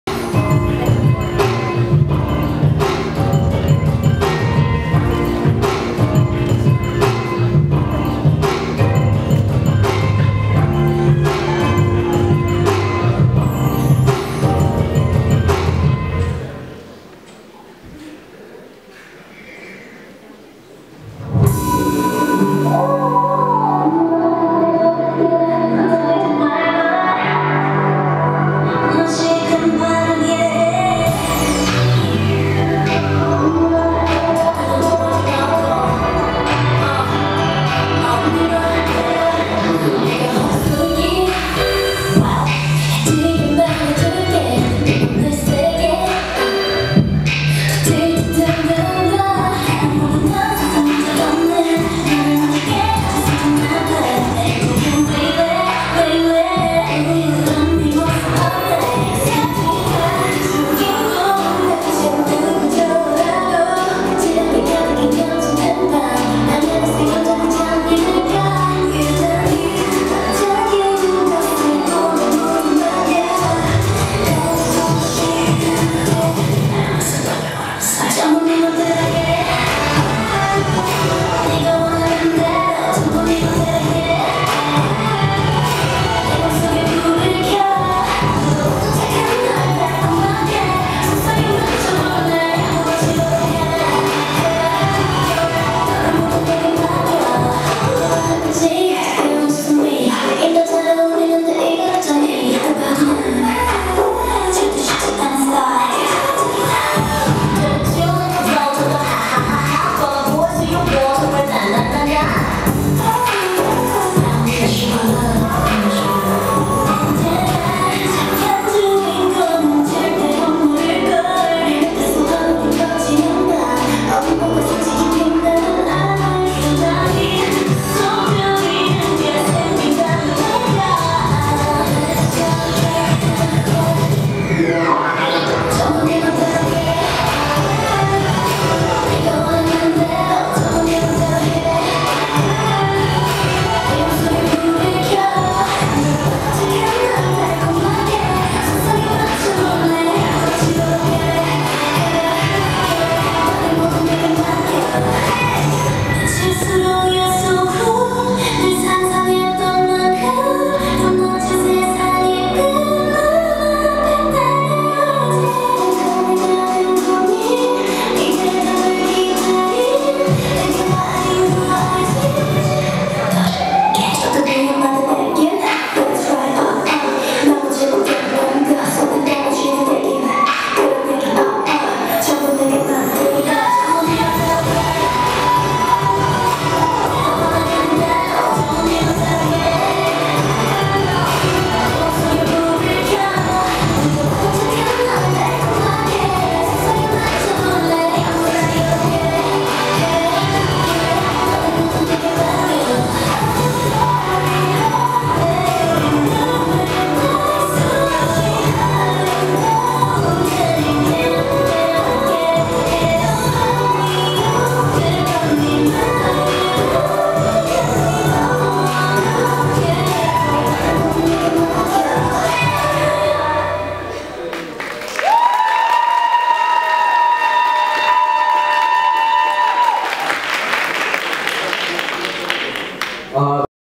S.R.A.M - Pristin V cover - FAP 2019 Festival of Asian Popular culture
FAP 2019. Festival of Asian Popular culture
Ярославль